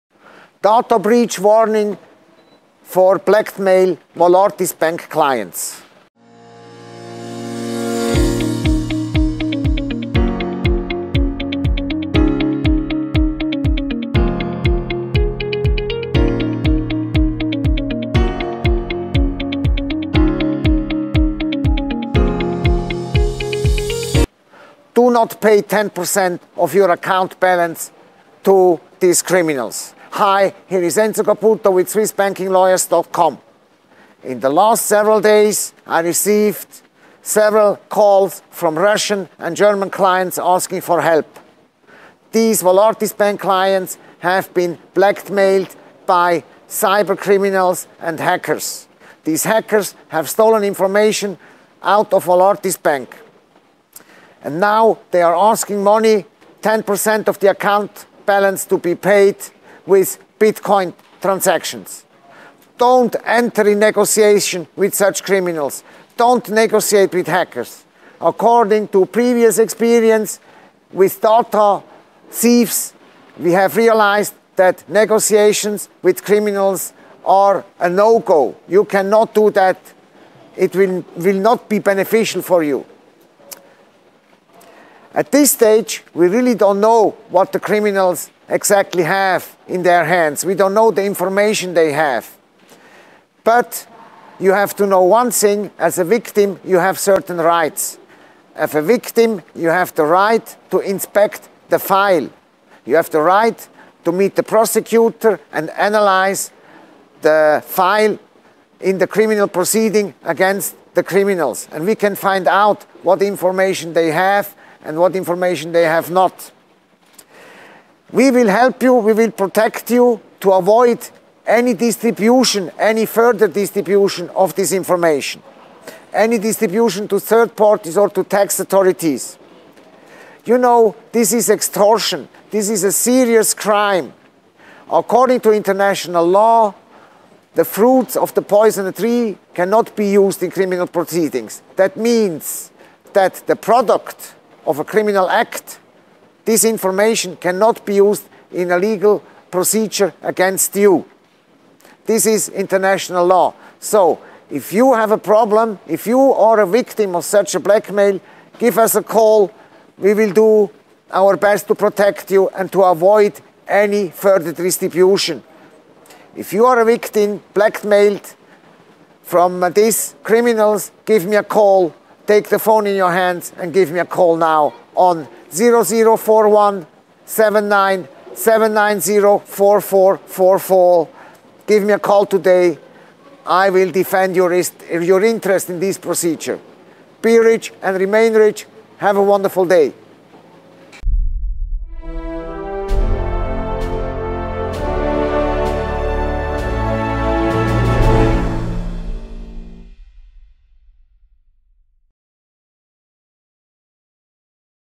Alert for blackmailed Valartis Bank Clients - Don't negotiate with Cyber Criminals and Hackers
In the last days I received several calls from Russian clients and German clients asking for help. The clients are blackmailed with 3 different emails received from Cyber Criminals and Hackers. They ask for 10% of the account balance to be paid with Bitcoins. Do not negotiate with Criminals. They will not cancel you from the list.

At this stage, we don’t know what data the hackers have in their hands. Criminal proceedings against the cyber criminals are pending. As a victim you have special rights. You have access to the criminal file and we can find out what they have in their hands.

We will protect you and we will stop any data disclosure to third parties, including authorities. This is extortion. Extortion is a serious crime. According to international law, the fruits of a poisoned try cannot be used in legal proceeding. The product coming out of a criminal act cannot be used against you.

Be rich and remain rich. Have a beautiful day.

⇨ Multilingual service
⇨ 24/7 support